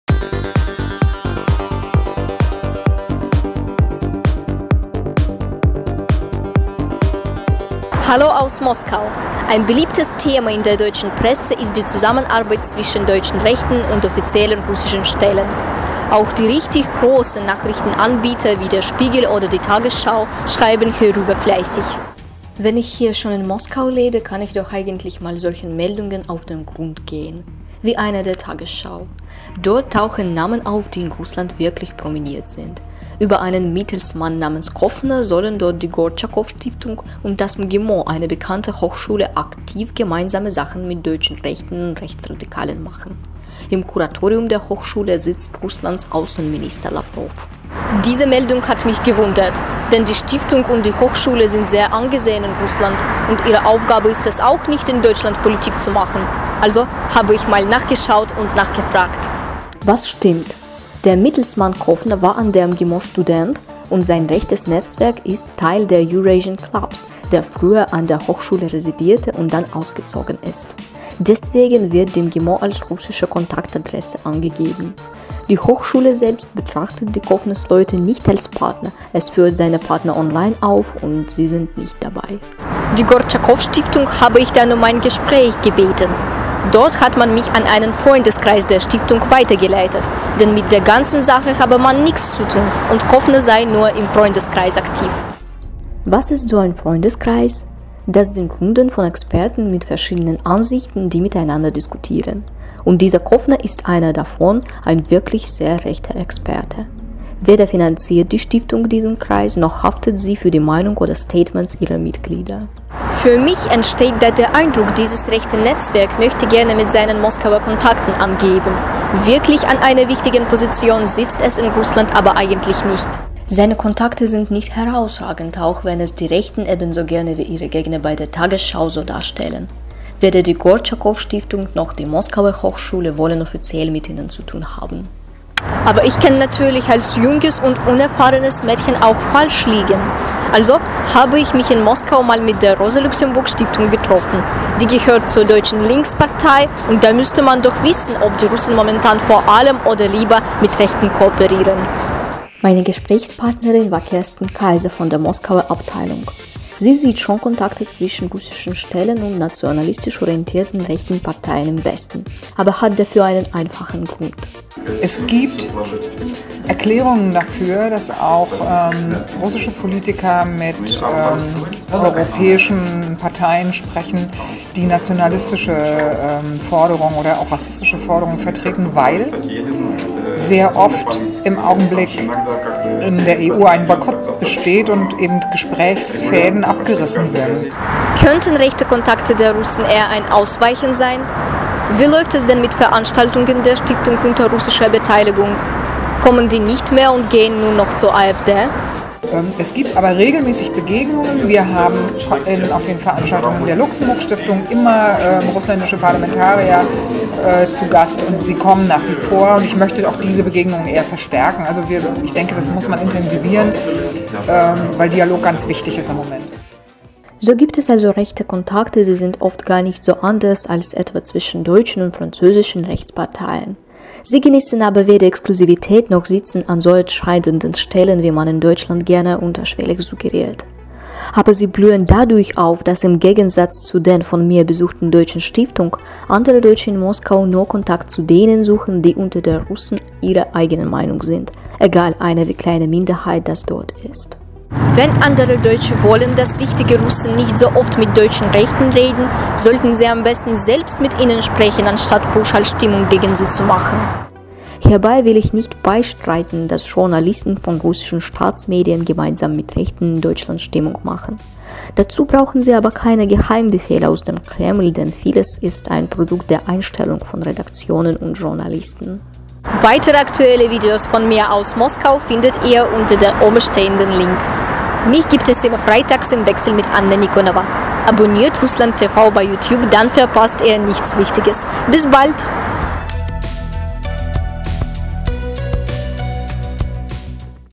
ARD-Nachrecherche: Russen & Rechte
Das vielleicht beliebteste Russlandthema der großen deutschen Medien sind aktuell neben allerlei bedrohlich-militärischem die russischen Kontakte (oder Kommandanten?) deutscher Rechter und Rechtsextremer.

All diese Berichte hat unsere junge Moskauer Politexpertin Julia Dudnik einmal zum Anlass genommen einer Veröffentlichung der Tagesschau genauer direkt vor Ort auf den Grund zu gehen, in der in der Tat renommierte russische Organsationen als Teil eines rechten Netzwerkes dargestellt wurden. Das Ergebnis offenbart einiges über jene Rechten ebenso wie ihre verschriebenen Gegner. Bei der Gelegenheit hat sich Julia gleich noch an fachkundiger Stelle in Moskau erkundigt, ob denn jetzt die russische Politik wirklich bevorzugt mit deutschen Rechtsparteien anbandelt. Hierbei ist sie nicht nur auf eine mögliche Erklärung für manchen Kontakt gestoßen, sondern hat sogar noch eine Strategie mitgebracht, die für AfD-Gegner in Russland vielversprechender ist, als das Absetzen zweifelhafter Pressemeldungen mit viel düsteren Vermutungen und wenig echte Fakten.

Alle Videos mit Julia Dudnik aus Moskau gibt es auf unserer Sonderseite zu ihrer Clipreihe